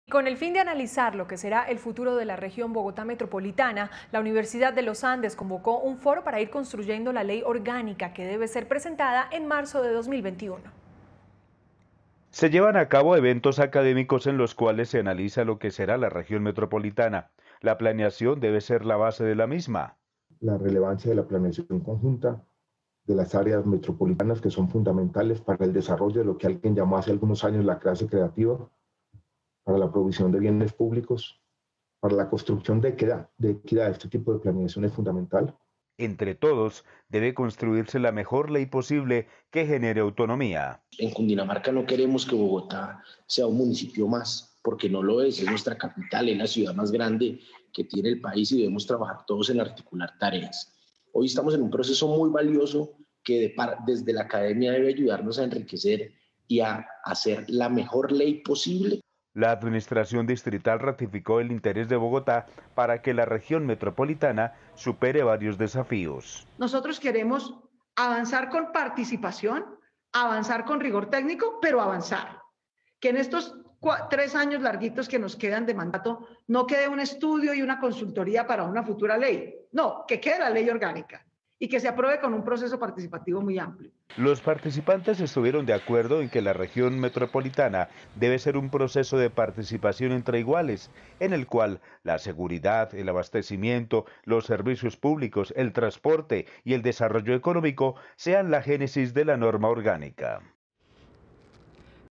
U. de los Andes realiza foro sobre la Región Metropolitana de Bogotá
NoticiasCapital | El propósito de este espacio es avanzar con participación en la formulación de esta ley orgánica.